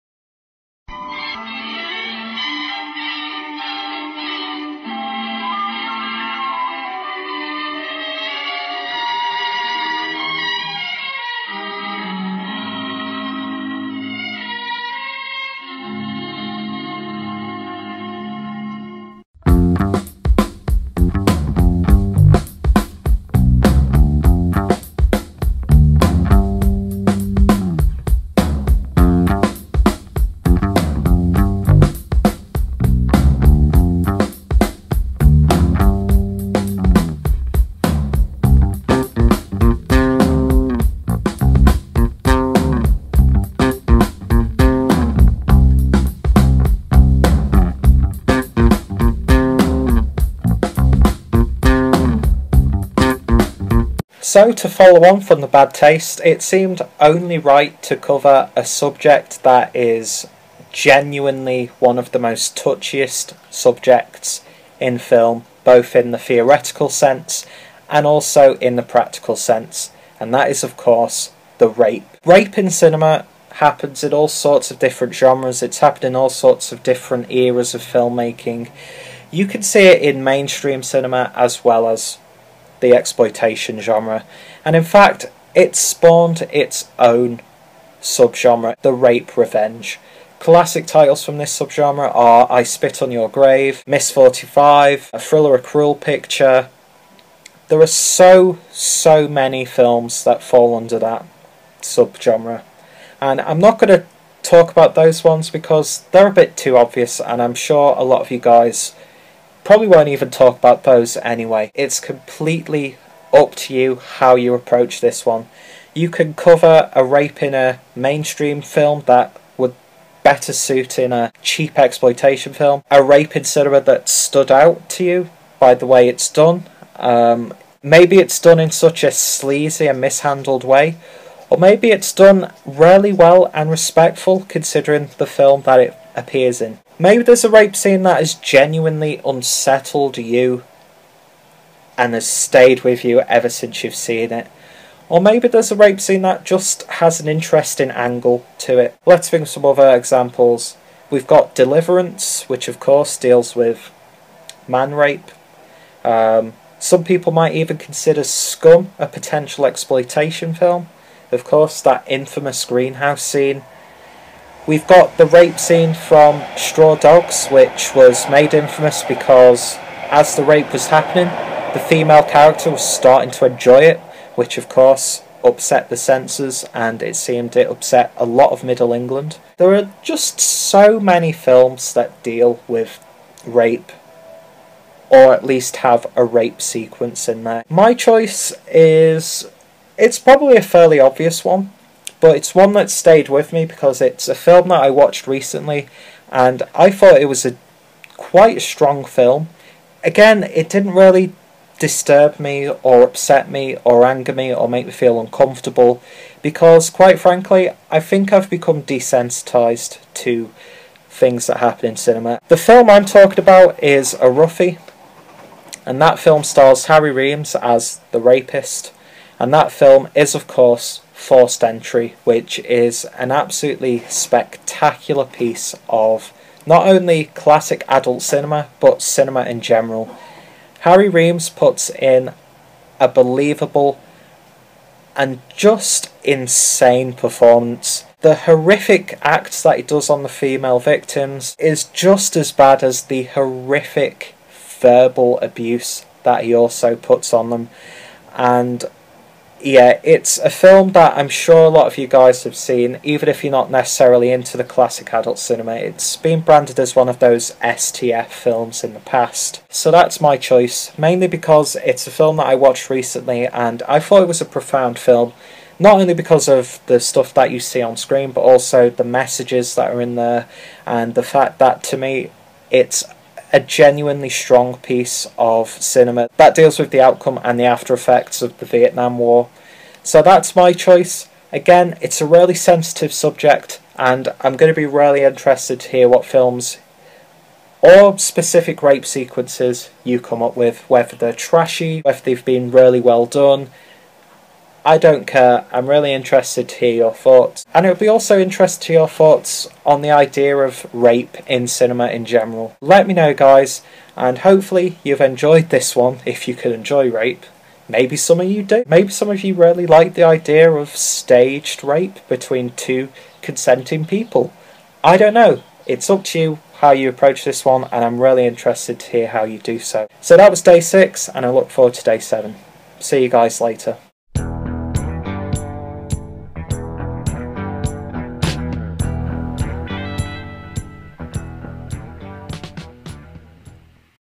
7 Days Of Exploitation: Day 6: Rape!
So we had Bad Taste yesterday. How do we follow that up? Well, with a very sensitive topic in and out of the world of film (understandably of course). Of course this is another one that will make you think and discuss about moments that could have been done in a very sensitive way or could have been done in such a trashy fashion. Maybe it was a scene that made you feel uncomfortable or maybe you are into this stuff on screen.

Stalk me:
Wish List: